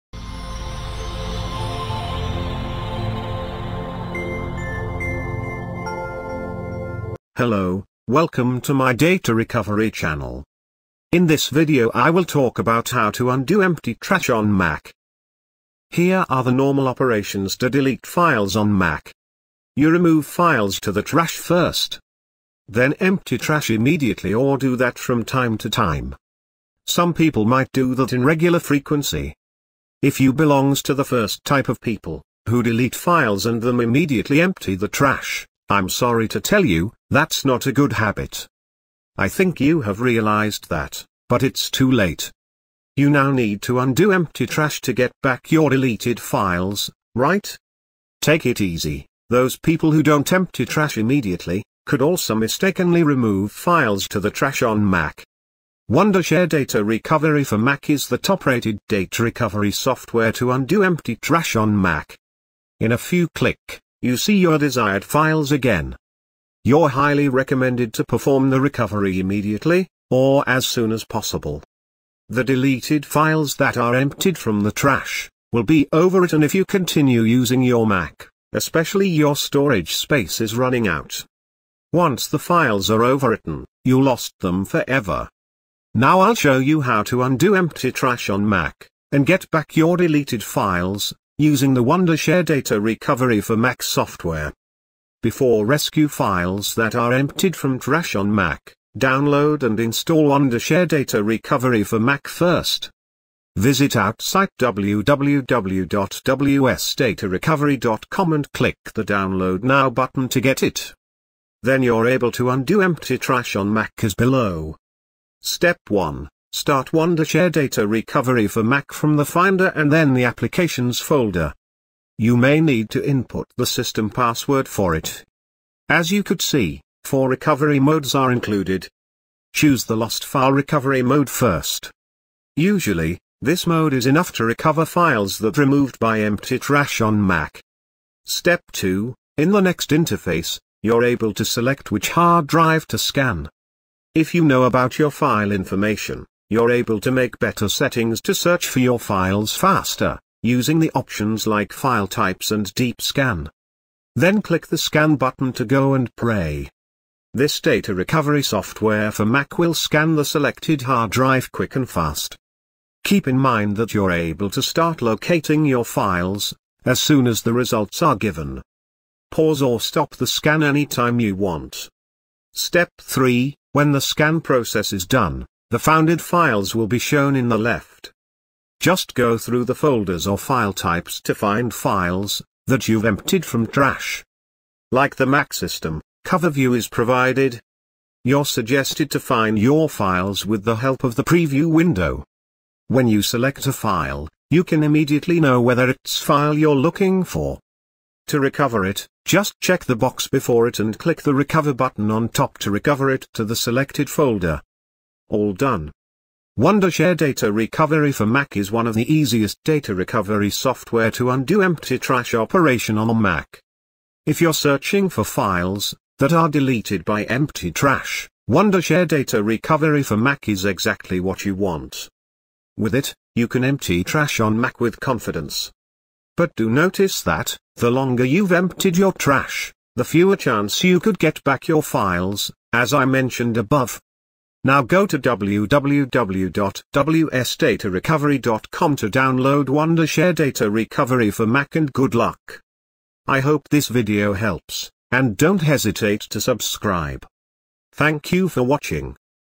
How to Undo Empty Trash on Mac Quickly and Easily
Do you delete files on Mac like this: remove files to the Trash and then empty Trash immediately. It's not a good habit. I think you have realized that,but it's too late. Now you need a reliable data recovery software like Wondershare Data Recovery for Mac to undo Empty Trash on Mac. See the detailed guide and good luck.

undo empty trash on mac
undo empty trash
undelete files on mac
recover emptied trash files